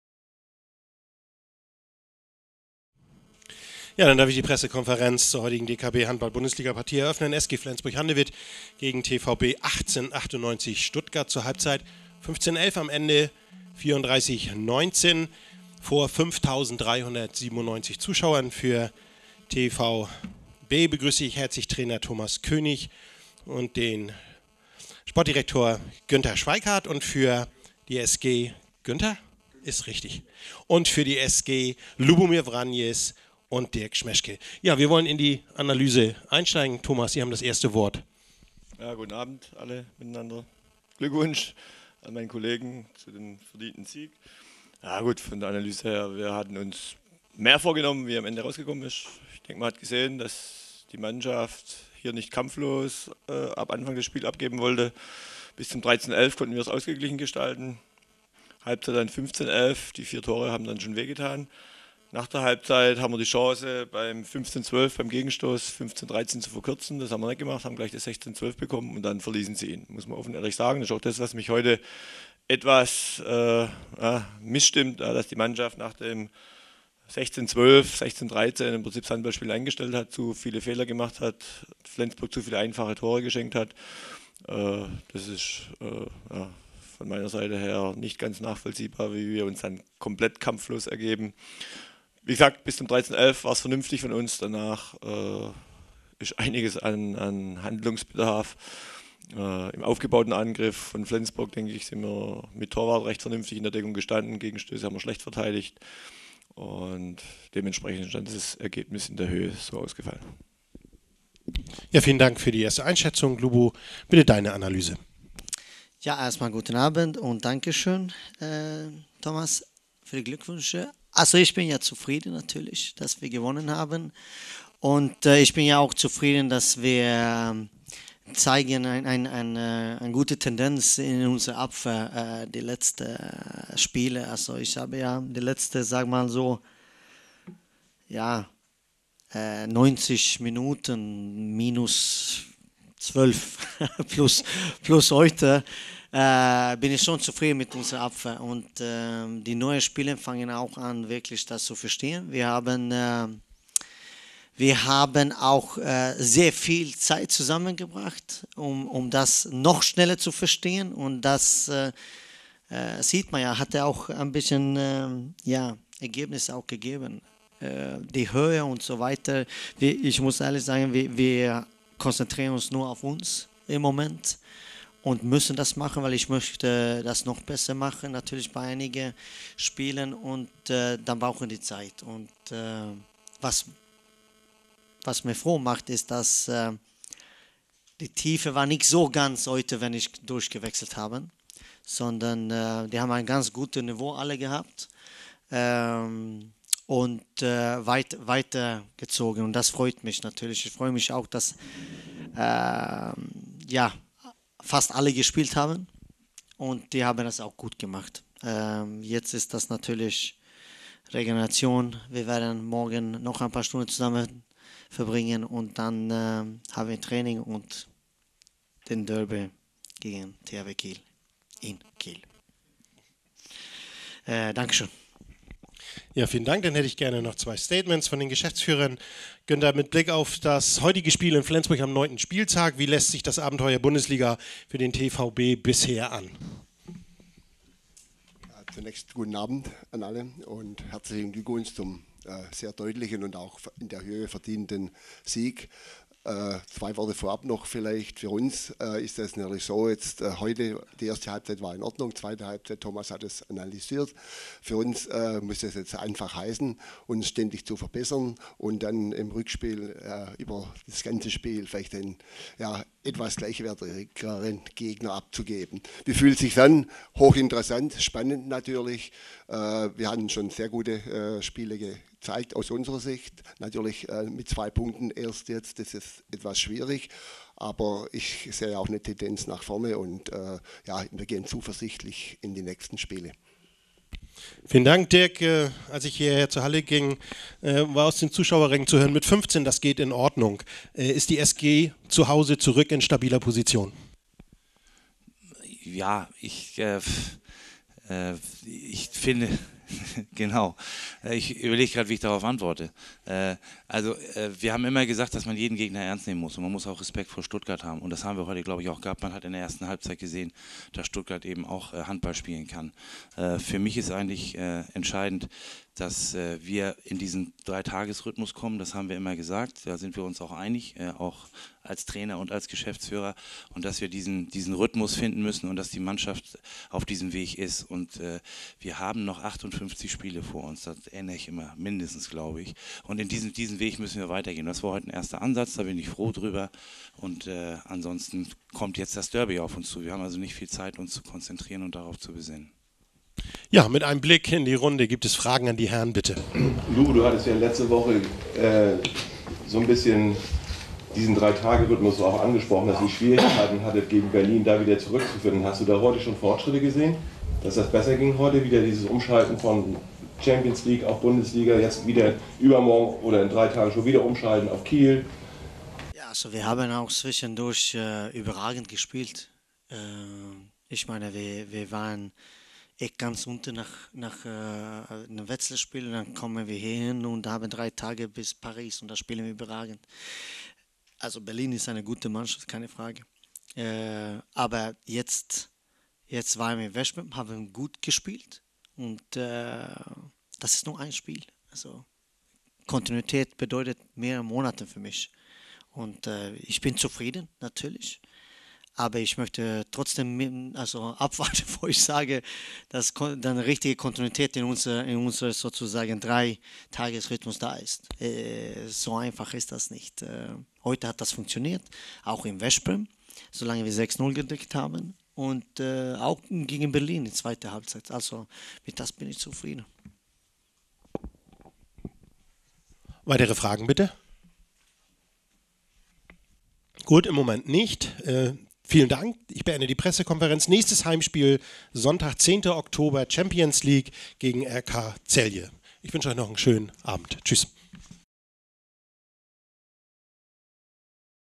Pressekonferenz nach dem Stuttgart Spiel
Tor-Festival in der FLENS-ARENA
Die SG Flensburg-Handewitt schlug am Abend den TVB 1898 Stuttgart mit 34:19 (15:11). Damit landete der Lokalmatador den dritten Heimsieg in der DKB Handball-Bundesliga und verbesserten ihr Konto auf 11:5 Punkte. (Ki.)